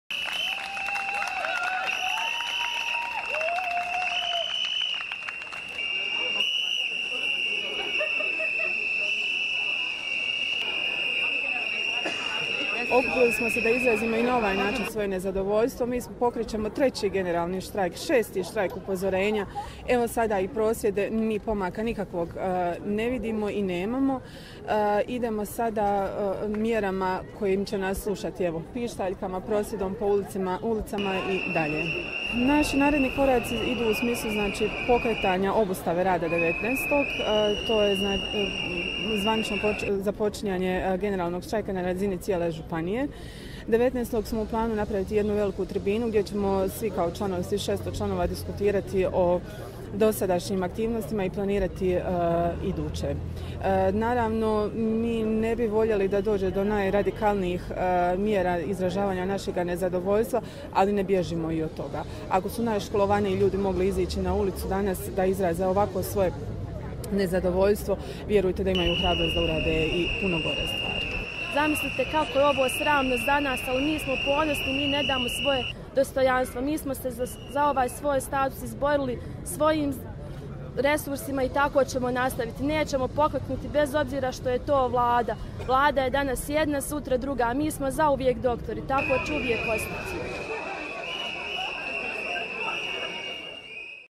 Mostar: Protesti doktora
Nezadovoljni su što Vlada HNK ne želi otpočeti zasebne pregovore o kolektivnom ugovoru.